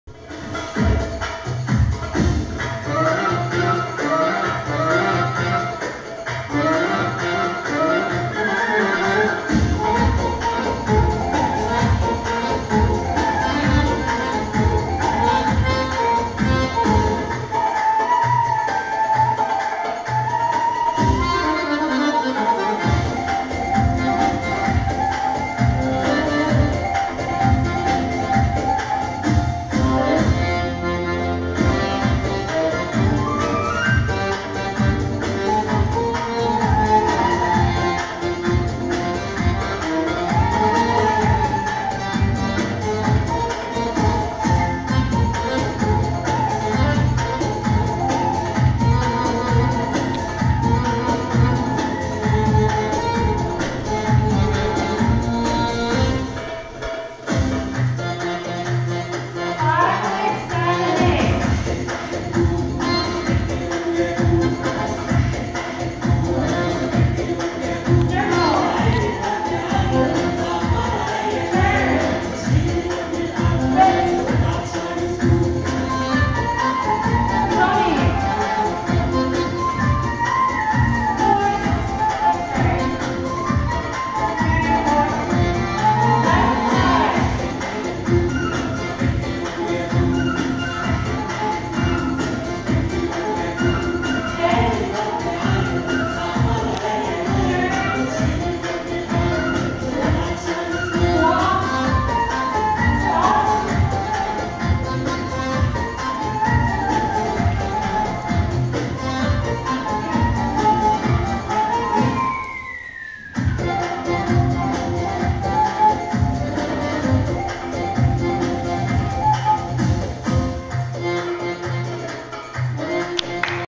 Bellydancing Class 4/5/2014 With Georgia Michelle
This was the last bit of the short routine Georgia shared with us in her belly dancing class on Saturday at Sacred Earth Temple. Really informative, lots to practice and drill! Thanks to Mirayah and Blue Mountain for hosting this!